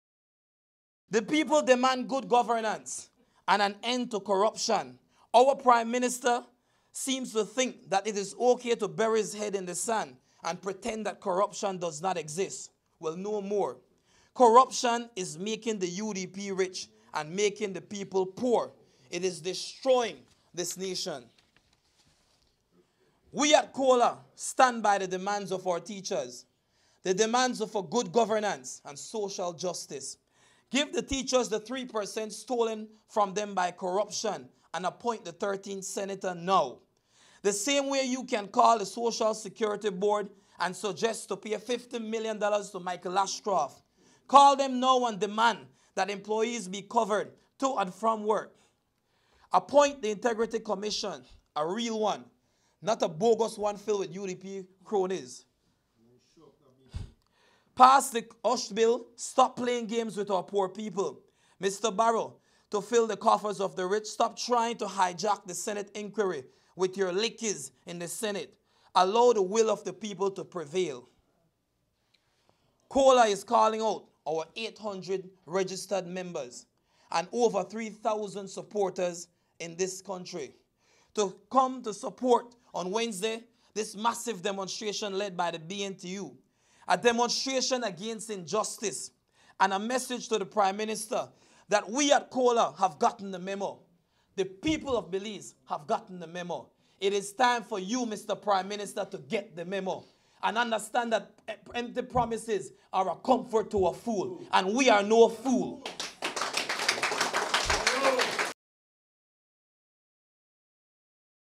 COLA Demands Good Governance Agenda be Implemented
For each of the demands by teachers for good governance, COLA is demanding G.O.B. to take action. Brackett says that government cannot continue to bury its head in the sand in the face of rampant corruption.